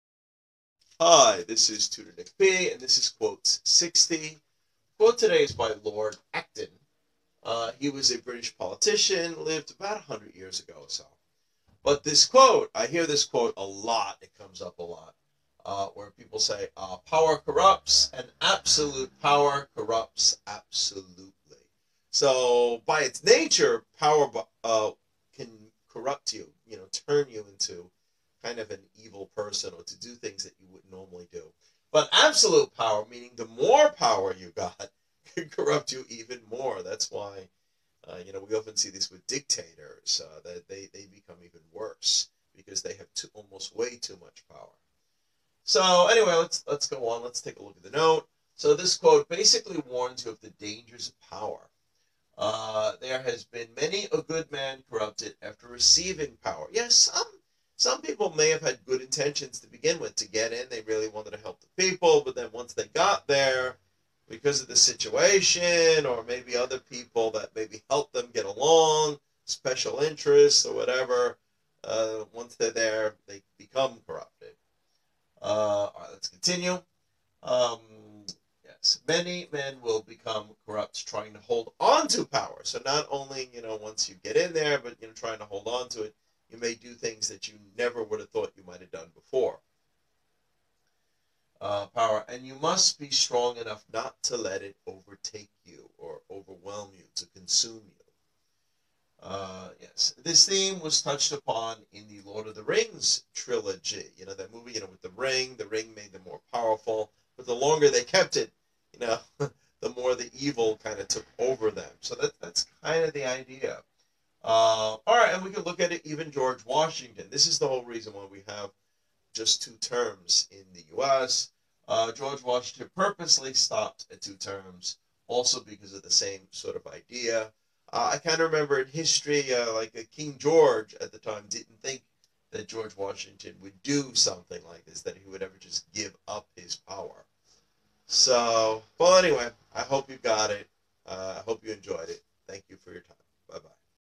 Tutor Nick P Quotes 60 - Lord Acton - Power Corrupts and Absolute Power Corrupts Absolutely
This quote basically warns you of the danger of power. It tends to have an evil nature of its owns that men who are in this position must be careful not to be overwhelmed by it.